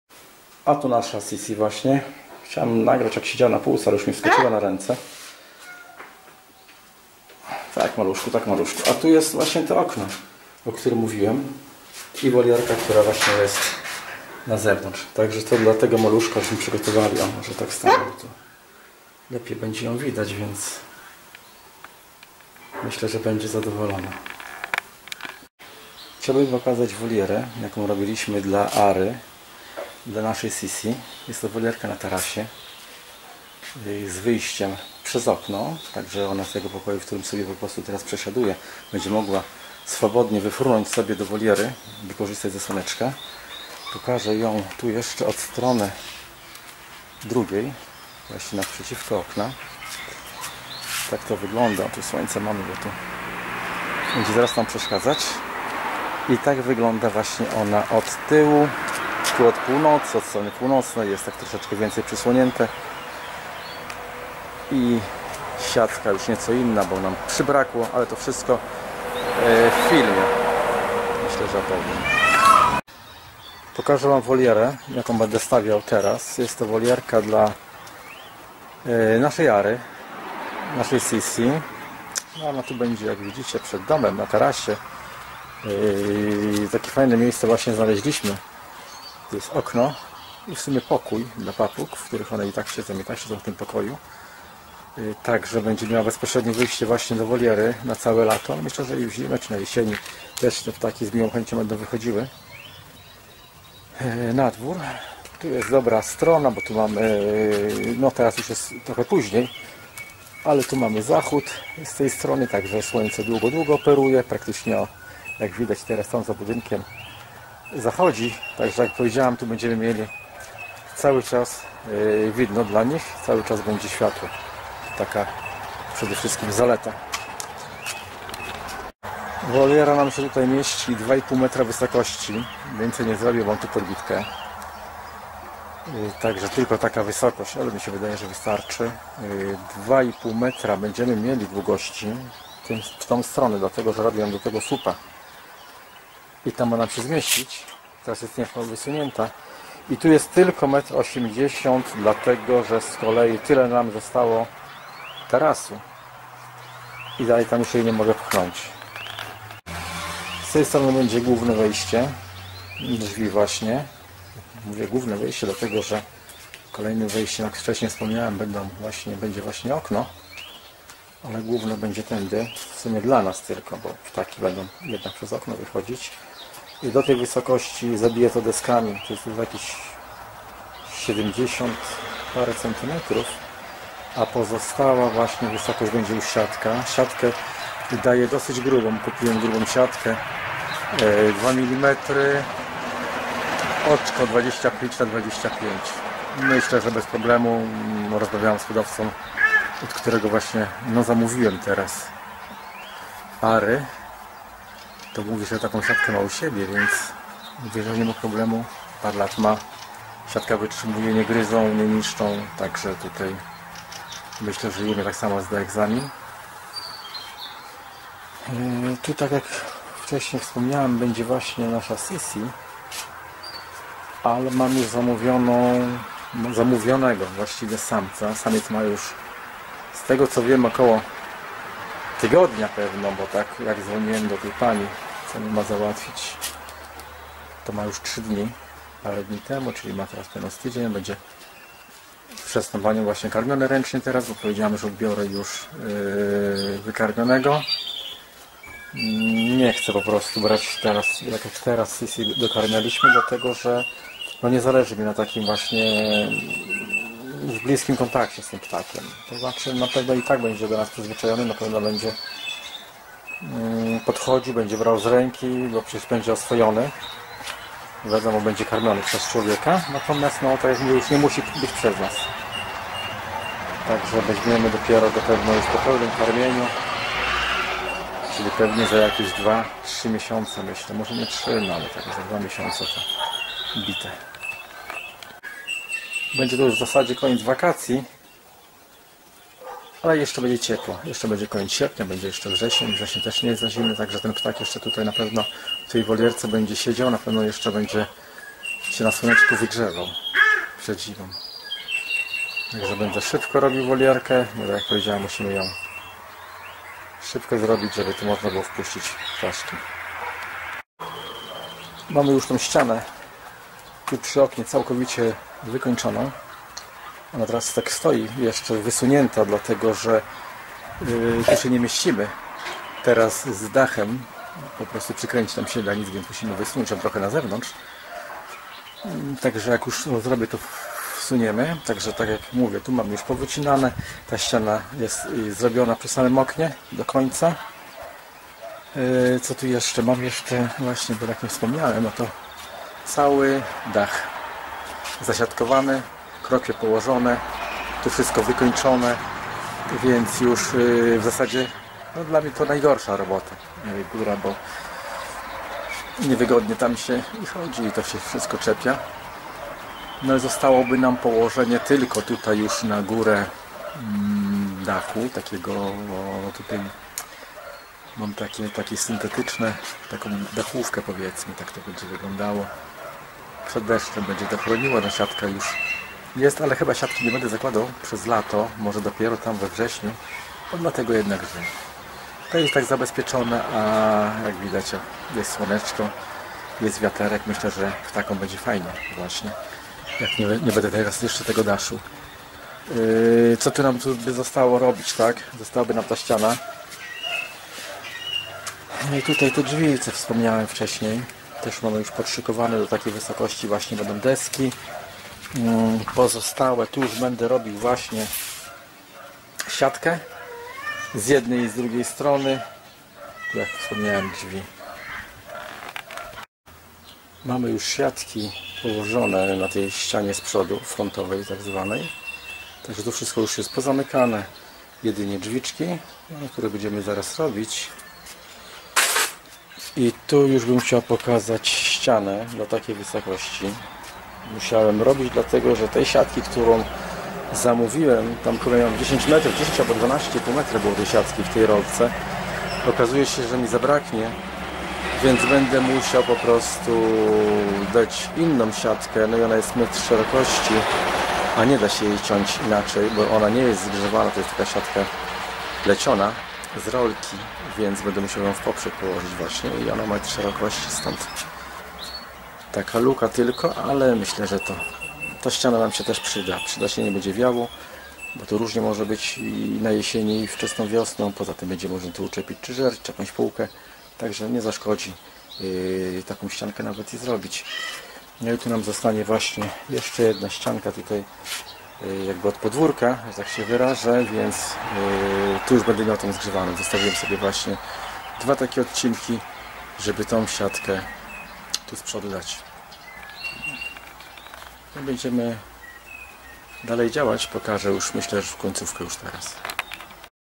jak zbudowałem wolierę dla Ary Ararauny-Sisi,how I built an aviary for macaw parrot,loro guacamayo
chcę pokazać jak zbudowałem wolierę dla papugi Ary Ararauny - Sisi,Stawiałem na tarasie przed domem, tak żeby mogła swobodnie wyfruwać z pokoju na taras, Mysle że takie rozwiązanie jest najlepsze dla papugi